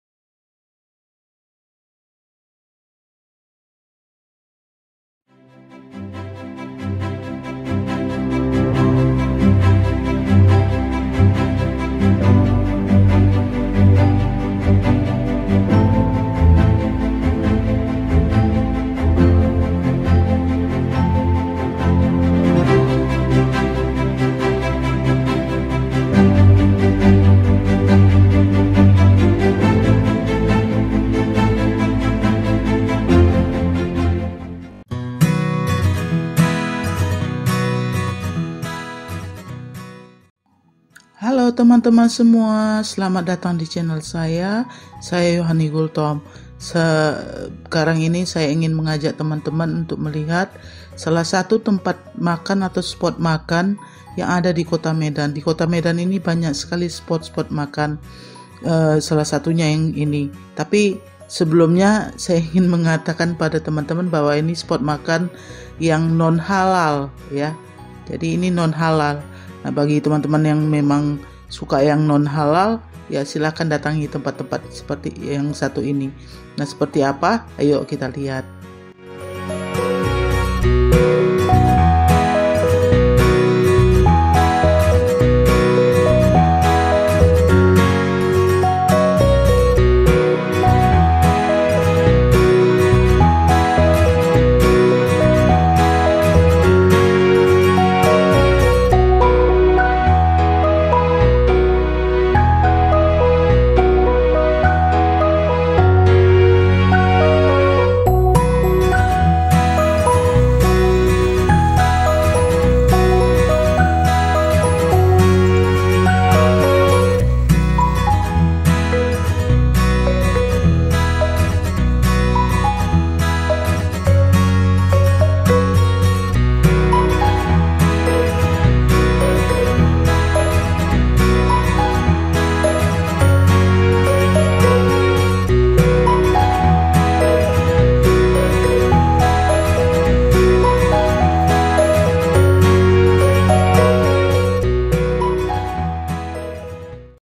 SPOT MAKAN DI KOTA MEDAN-GOLDEN
Hai. Ini adalah salah satu spot makan yang ada di kota Medan. Nama daerahnya adalah GOLDEN yang terletak di jln. S, Parman Medan. Tapi ini tempat makan yang non-halal. Semoga video ini menambah rekomendasi buat teman-teman yang ingin menikmati makanan non-halal. Selamat menyaksikan. God bless.